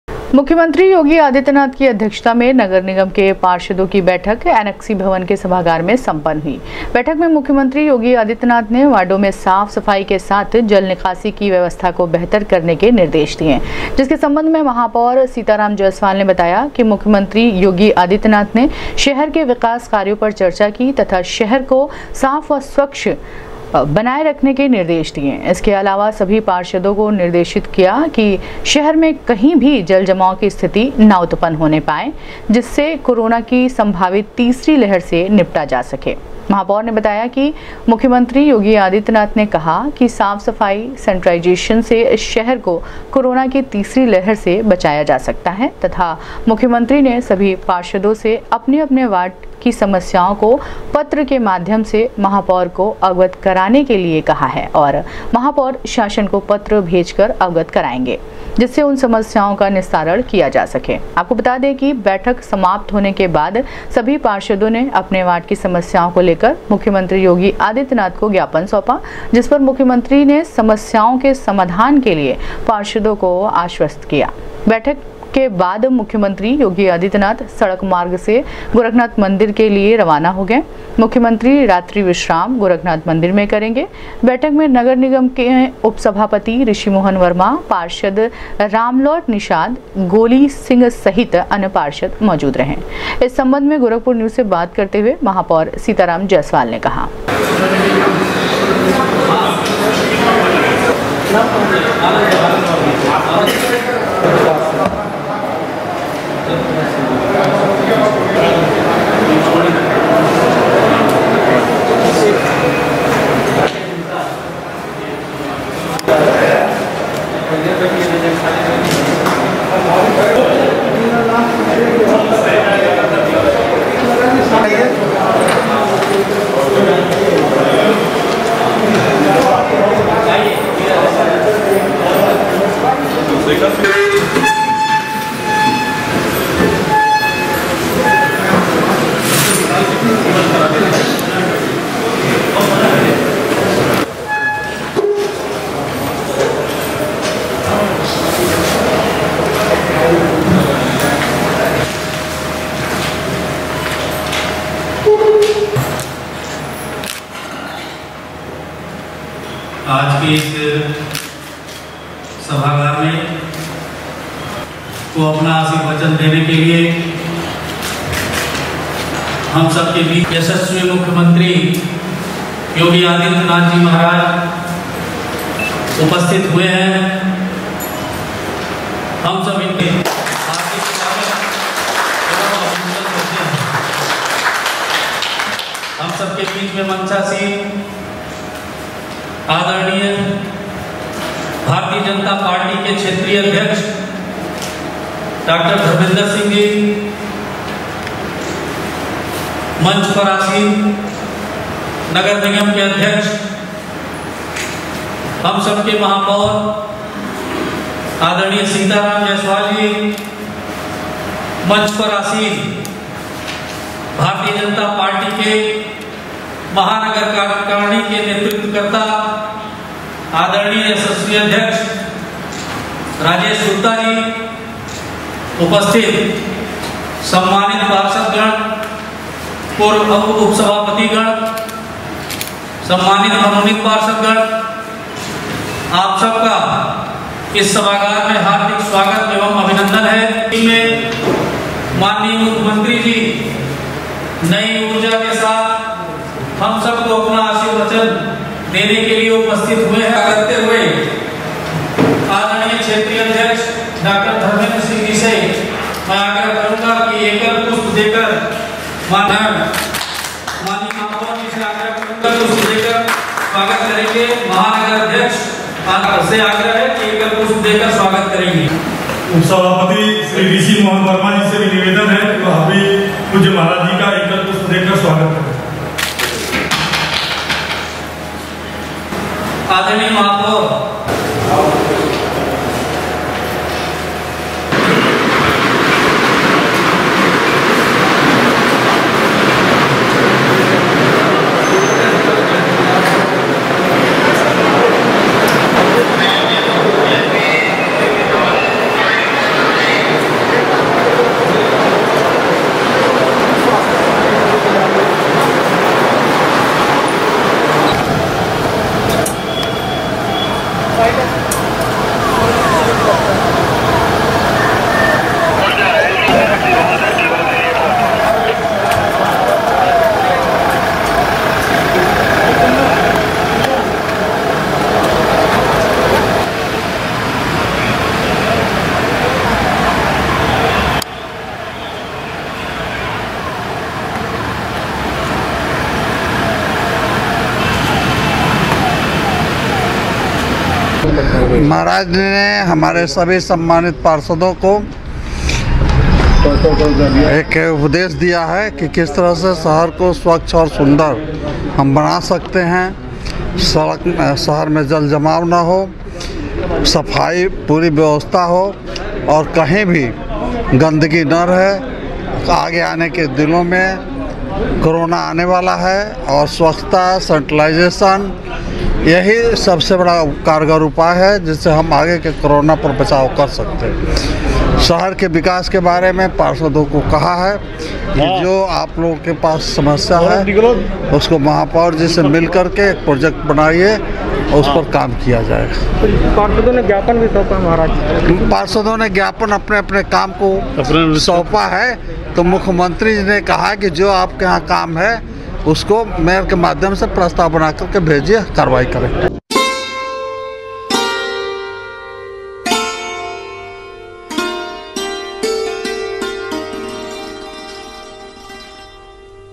मुख्यमंत्री योगी आदित्यनाथ की अध्यक्षता में एनेक्सी भवन के सभागार में सम्पन्न हुई नगर निगम के
मुख्यमंत्री योगी आदित्यनाथ की अध्यक्षता में एनेक्सी भवन के सभागार में सम्पन्न हुई नगर निगम के पार्षदों की बैठक,वार्डो के साफ,सफाई तथा जलजमाव को दूर करने के दिये निर्देश